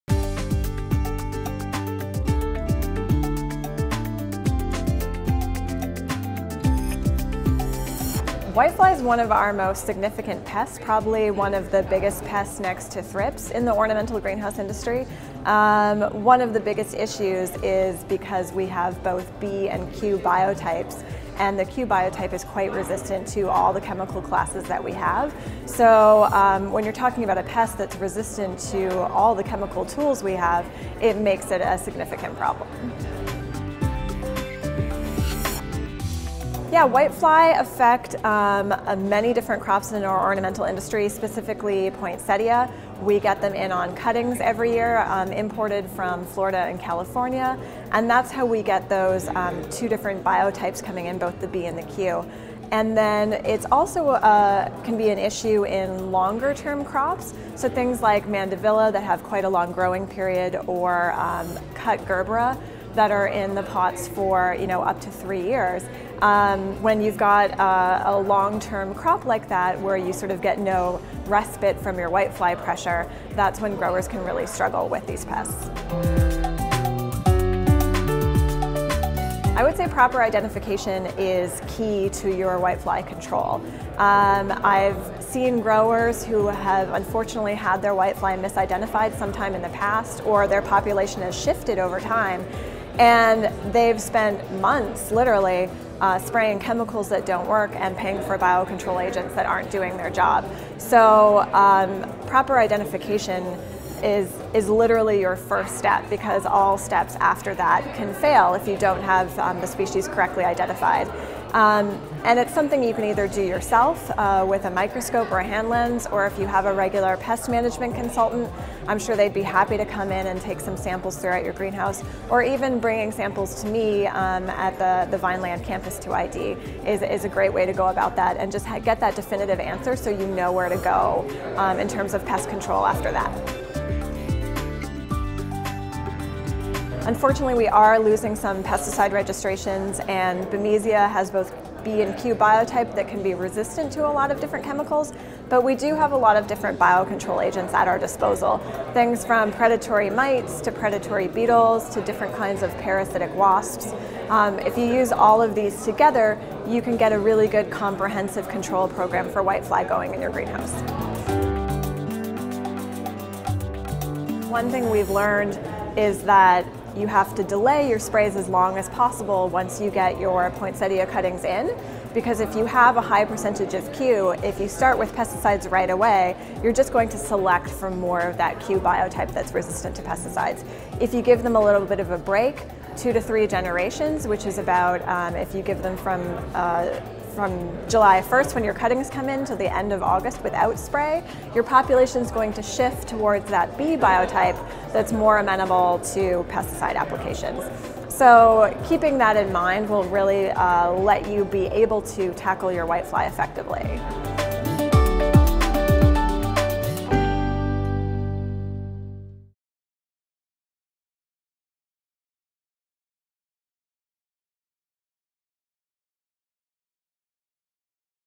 Whitefly: Basics, biology, species of concern and identification
Speaker: Dr. Sarah Jandricic, OMAFRA Greenhouse Floriculture IPM specialist.
Correctly identifying the whitefly species you’re dealing with and understanding its basic biology is the first step to successful whitefly control. This presentation will go over the common species found in greenhouses, the crops they are typically associated with, and the differences/similarities in their biology and management.